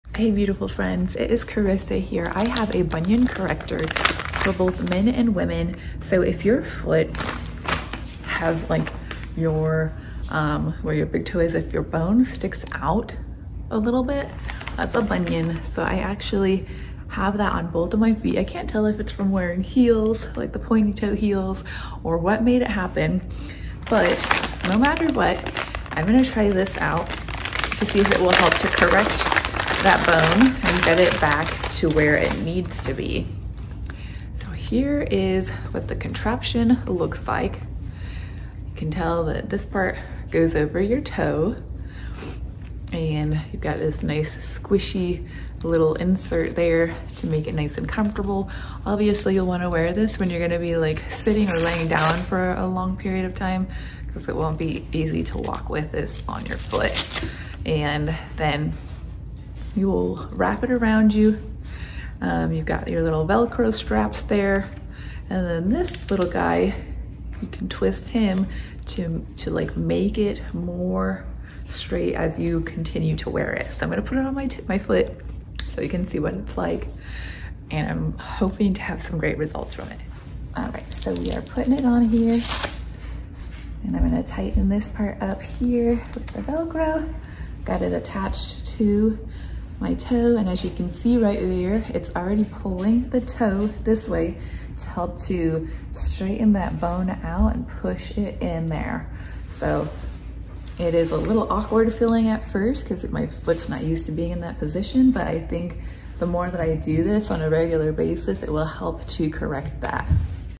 Using a bunion corrector to fix my bone structure
Does this bunion corrector really work? I am giving it a try to get my bone lined up correctly.